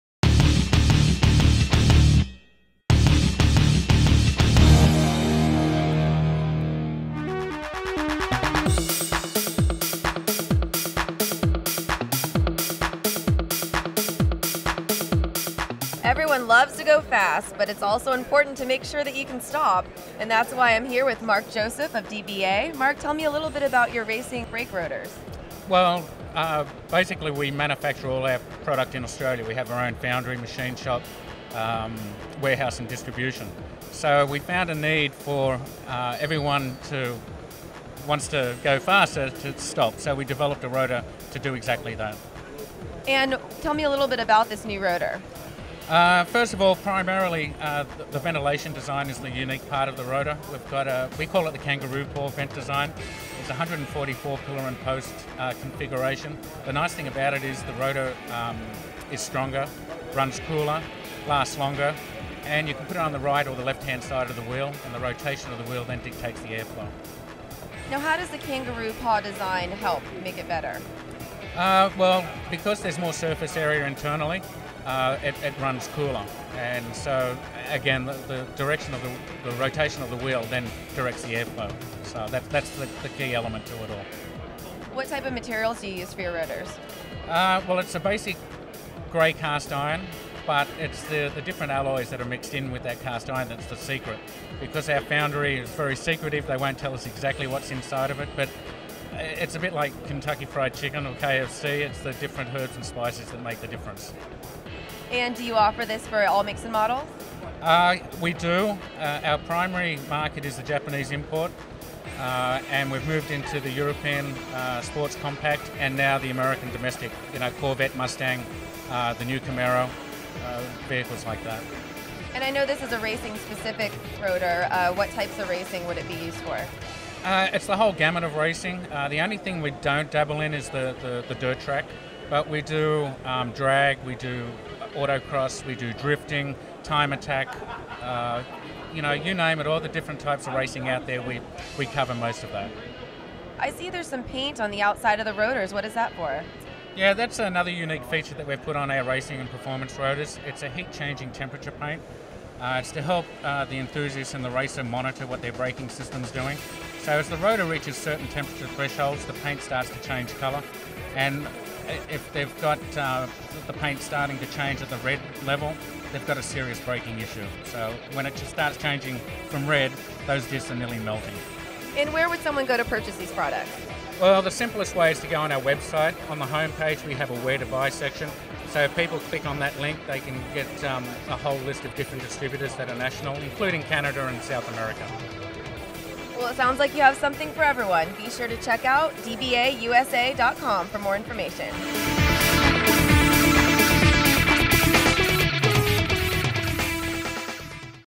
Performance Racing Industry Trade Show - PowerTV & Disc Brakes Australia
PowerTV's Melissa Lawrence talking to Mark Joseph of Disc Brakes Australia at the 22nd Performance Racing Industry Trade Show.